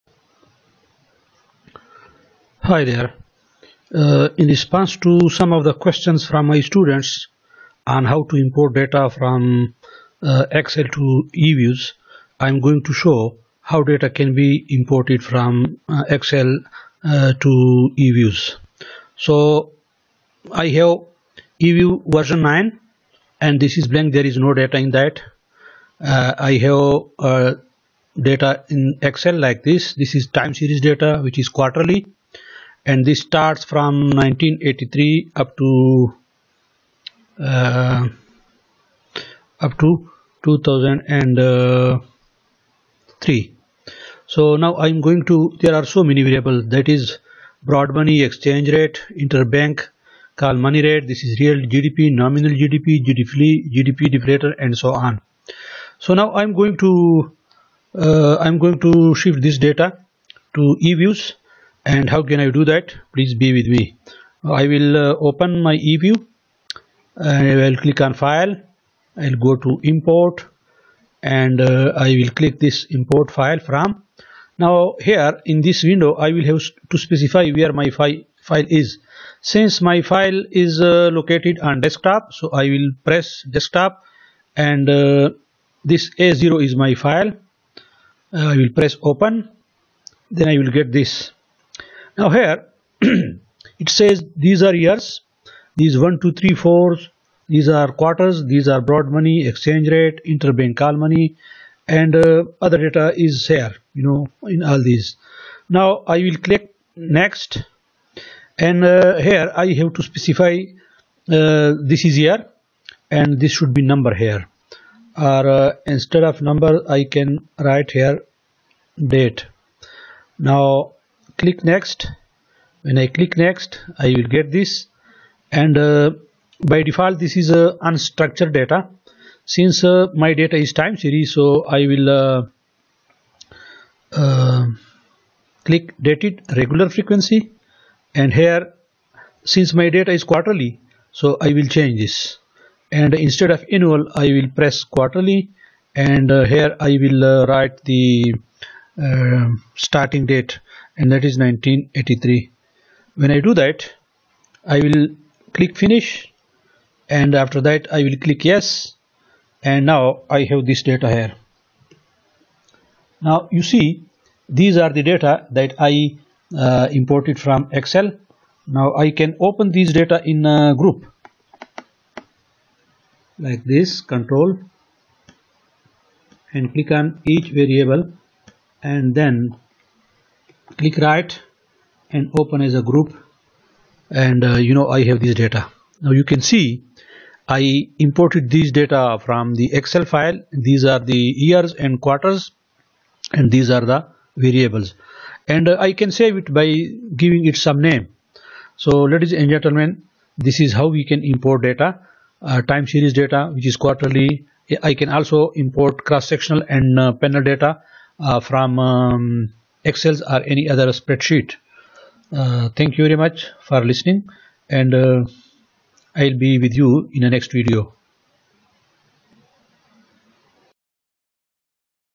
28 How to import Time Series data from Excel to E Views
This video shows how to import time series data from MS Excel to E-Views 9.